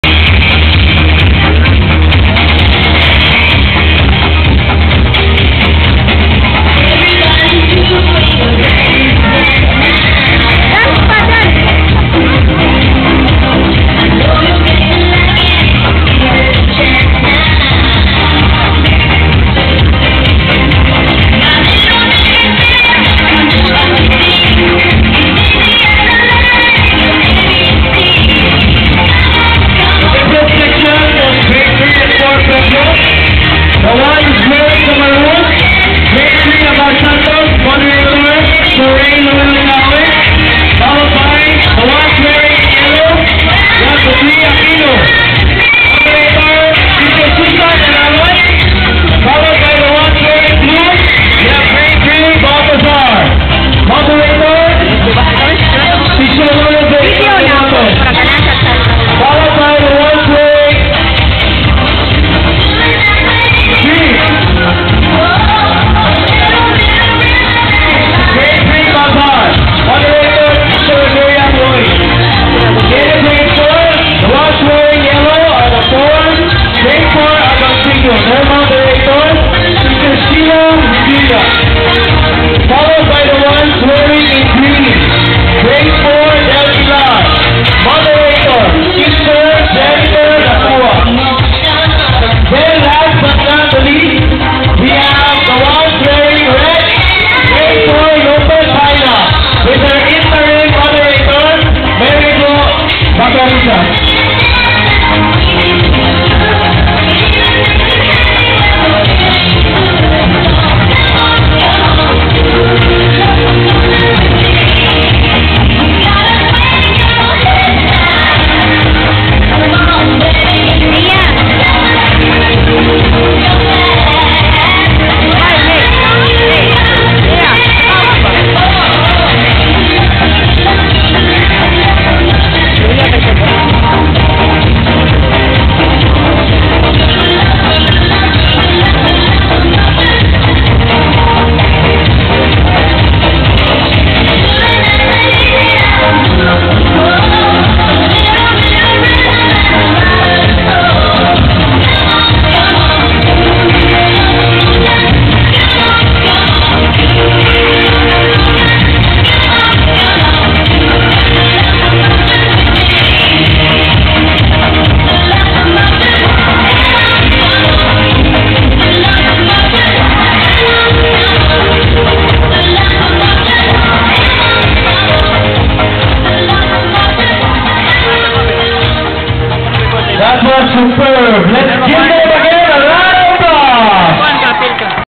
MAIA FRANCES MEDRANO
Grade 3 - Malvar FIELD DEMONSTRATION.  We were late because we came from check-up because Maia got sick.